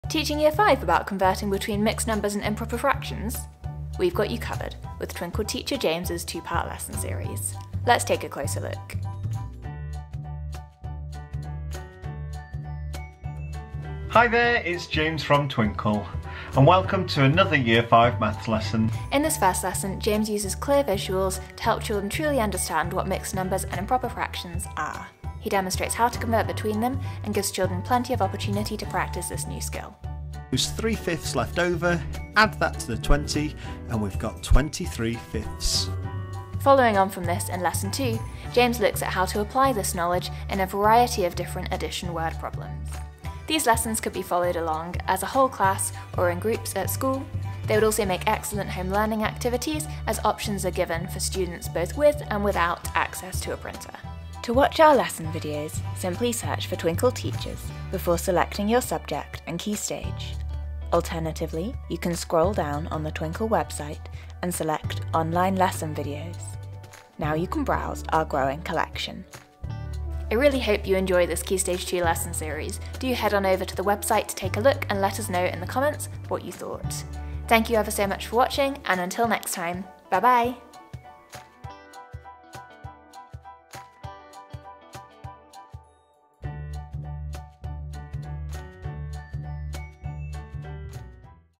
Year 5 (Ages 9-10) Fractions Lesson Videos
In this video, Twinkl Teacher Rae shares an overview of our Year 5 (Ages 9-10) Converting Mixed Numbers and Improper Fractions Video Lessons. Watch the complete Lesson Videos here:

In this pair of lesson videos, Twinkl Teacher James recaps fractions with children, and uses pictorial representations to introduce children to mixed numbers and improper fractions. He looks at how to convert between the two, and provides contextual practice within word problems.

0:00 Introduction
0:13 Lesson 1
0:35 Lesson 2
0:43 How to Use and Where to Find